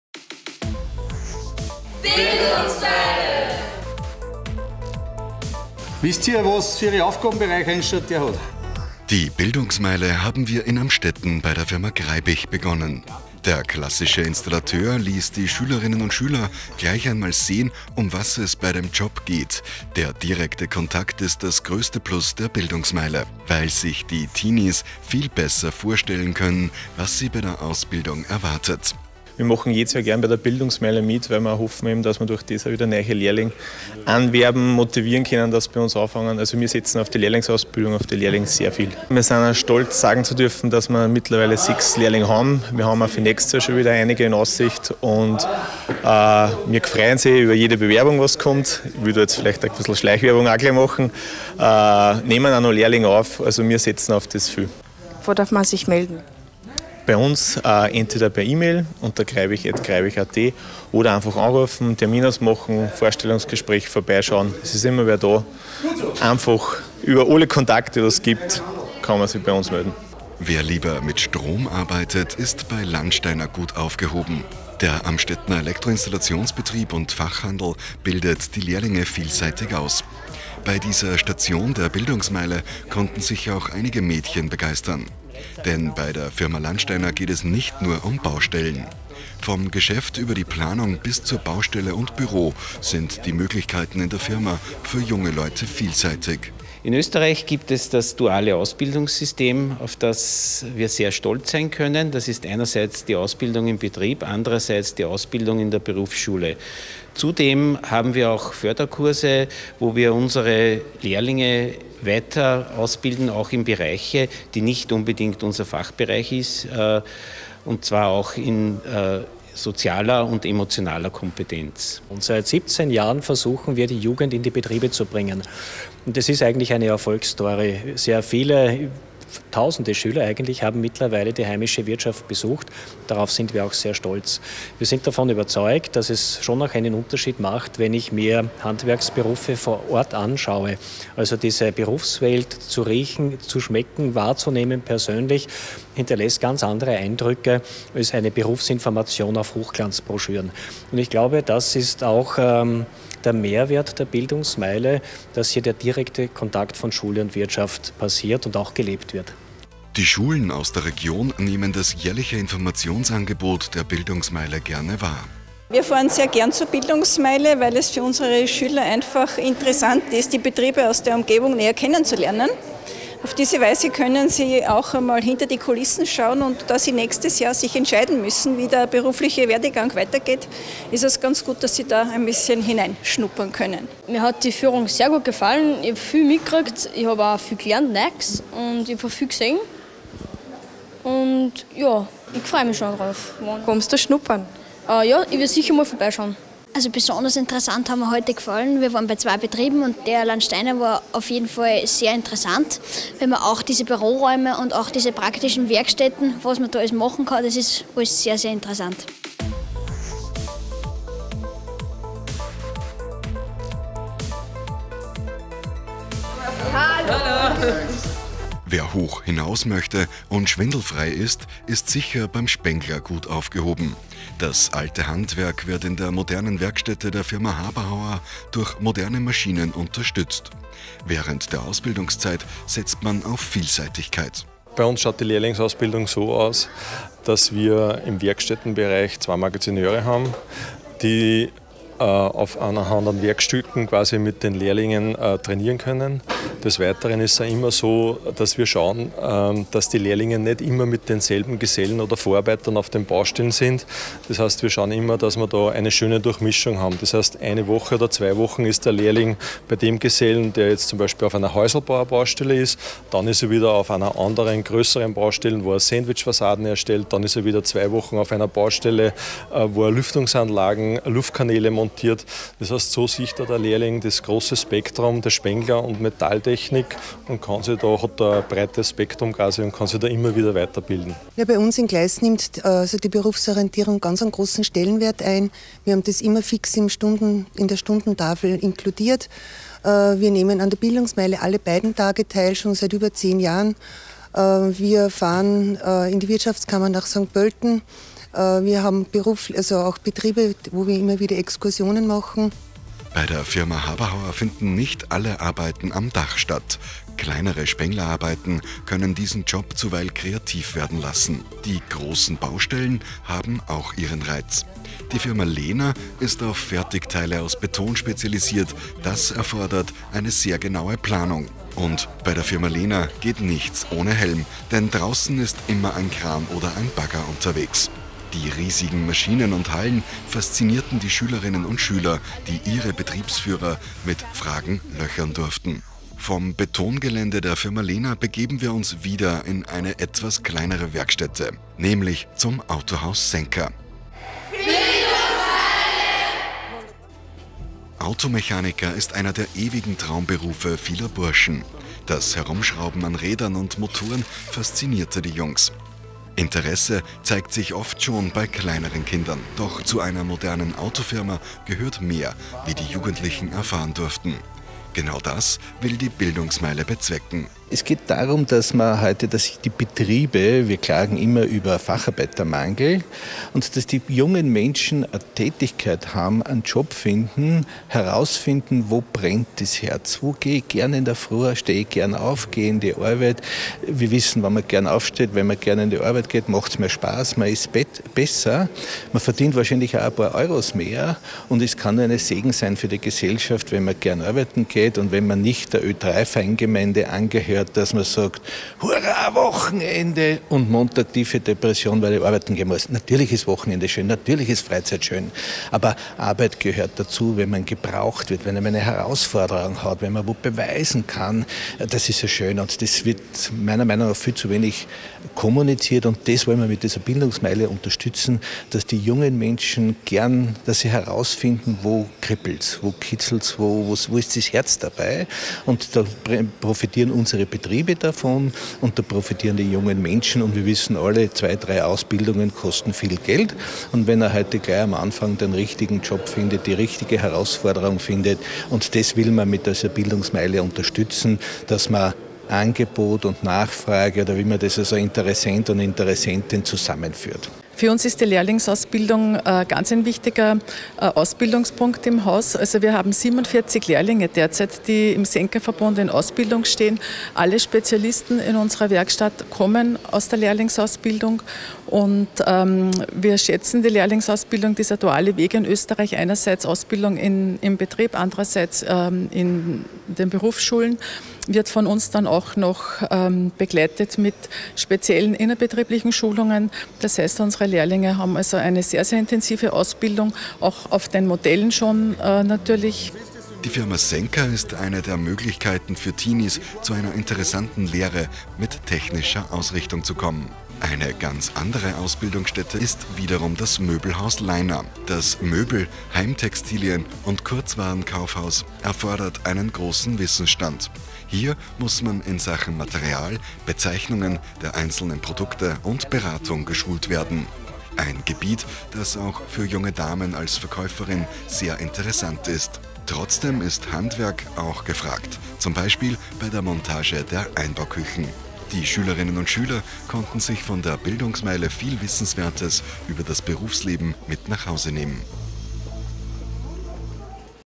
Bildungsmeile 2019
Die Firmen-kennlern-Meile in Amstetten haben wir bei der Firma Greibich begonnen. Der klassische Installateur ließ die Schülerinnen und Schüler gleich einmal sehen, um was es bei dem Job geht. Der direkte Kontakt ist das große Plus der Bildungsmeile, weil sich die Teenies viel besser vorstellen können,  was sie bei der Ausbildung erwartet